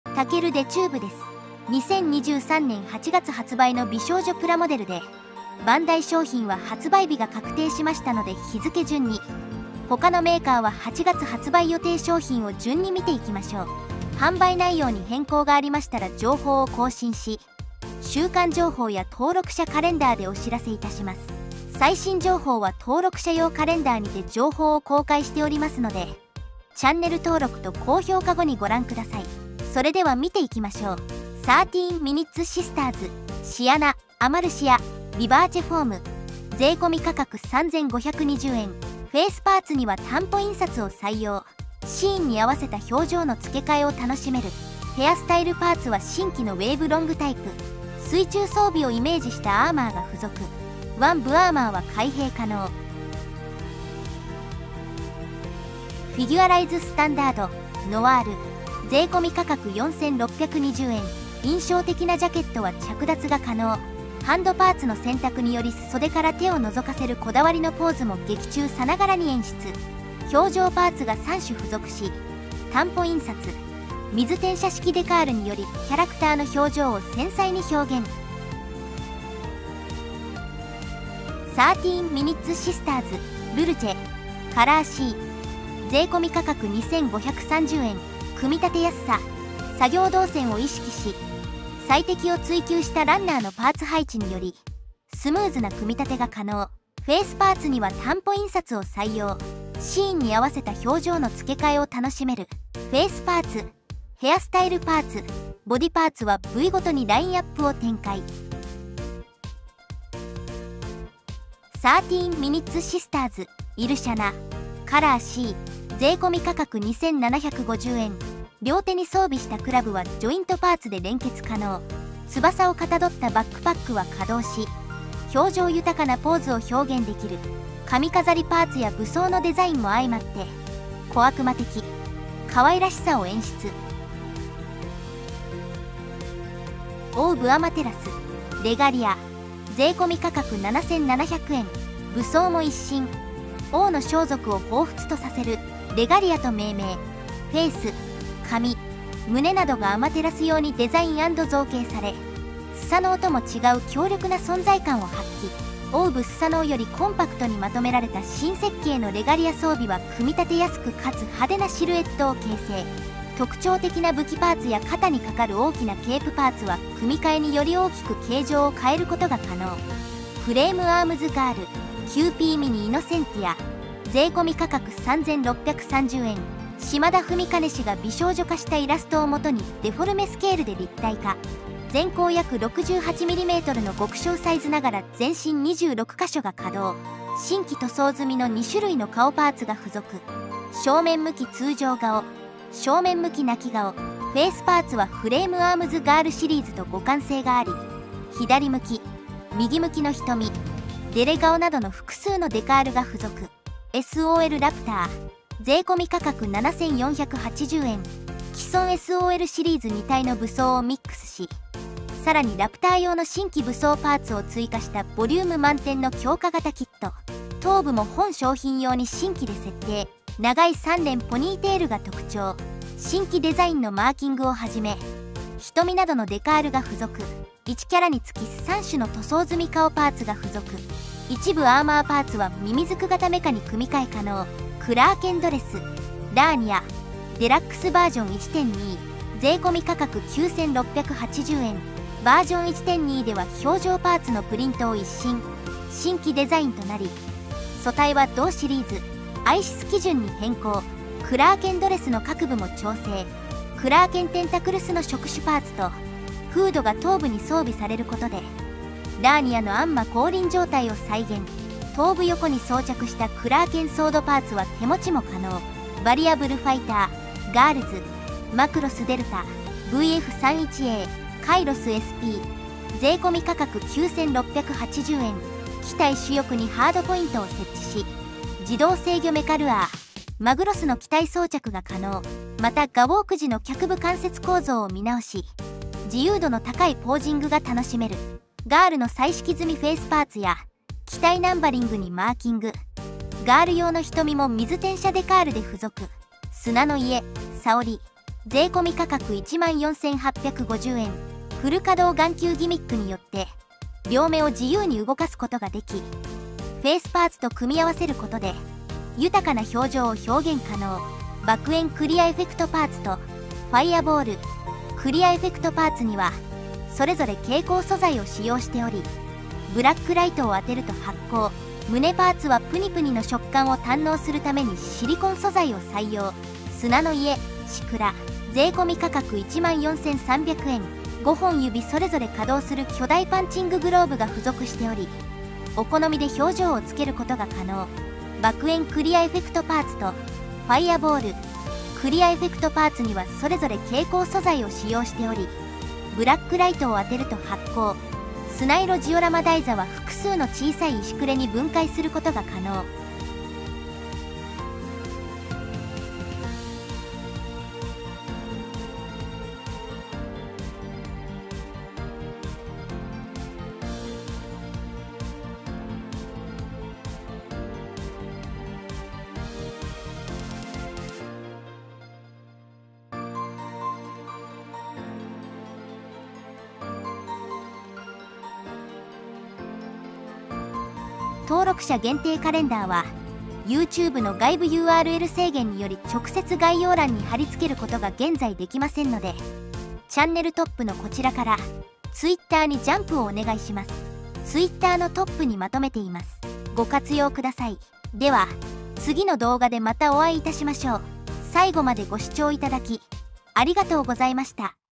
「月刊美少女プラモ情報」2023年８月発売・一部メーカー日付確定分反映 概要欄に商品一覧を置いています。
８月７日
30MS SIS-Ac19b シアナ=アマルシア（ヴィヴァーチェフォーム）　3,520円
30MS オプションヘアスタイルパーツVol.3 全4種　2,640円
30MS オプションヘアスタイルパーツVol.5 全4種　2,640円

　８月１１日（祝・金）
Figure-rise Standard ノワール　4,620円

　８月24日（木）
30MS SIS-A00 ルルチェ［カラーC］ 2,530円

　８月26日（土）
30MS SIS-M00 イルシャナ[カラーC]　2,750円
30MS オプションパーツセット8(スカウトコスチューム)[カラーC]　1,650円
30 MINUTES SISTERS 水転写式デカール 汎用2　660円

　８月３１日（木）
30MS オプションボディパーツタイプG02［カラーC］ 880円
30MS オプションボディパーツタイプS03［カラーC］ 880円
30MS オプションボディパーツタイプS04［カラーC］ 880円

皇巫 アマテラス レガリア　7,700 円（税込）
作品名：メガミデバイス

フレームアームズ・ガール Qpmini イノセンティア　3,630 円（税込）
作品名：フレームアームズ・ガール シリーズ

SOLラプター　7,480 円（税込）

ガバナー バンプアップ・エクスパンダー　3,960 円（税込）
作品名：ヘキサギア
ガバナーウェポンズ ガトリングブレード　2,420 円（税込）
メカサプライ01 フレキシブルアームA　1,100 円（税込）
作品名：M.S.G
ヘヴィウェポンユニット16 オーバードマニピュレーター　1,650 円（税込）
作品名：M.S.G

アルファマックス
SKYTUBE DarkAdvent Krakendress ラーニア DX Ver.1.2　9,680円

蝸之殻SNAIL SHELL
砂の家-砂織(さおり) 1/12 完成品アクションフィギュア[蝸之殼Snail Shell] 14,850円
砂の家-時桜(しくら) 1/12 完成品アクションフィギュア[蝸之殼Snail Shell] 14,300円

アオシマ
V.F.G. マクロスΔ VF-31A カイロスSP
価格9680税込

30MS SIS-T00 リリネル［カラーA］  2,750円→９月延期